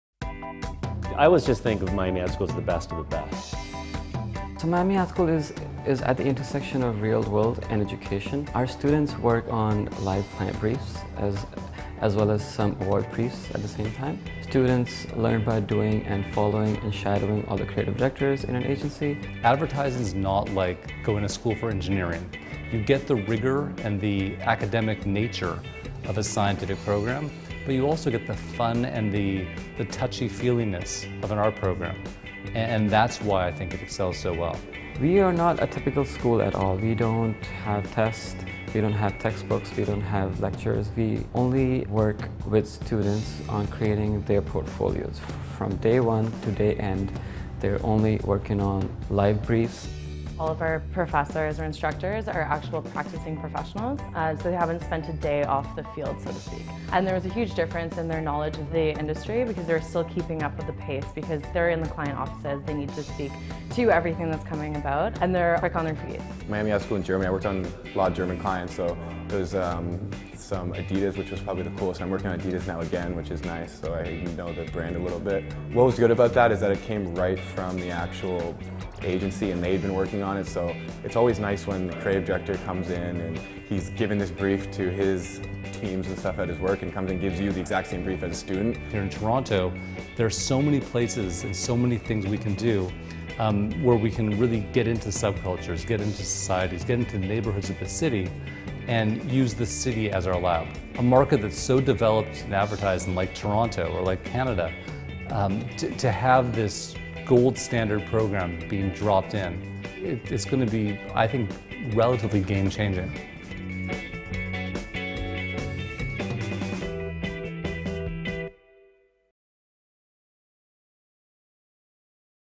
What to expect from Miami Ad School Toronto!
Marketing Magazine recently published a video feature highlighting why is MAS known as the most awarded advertising school in the world.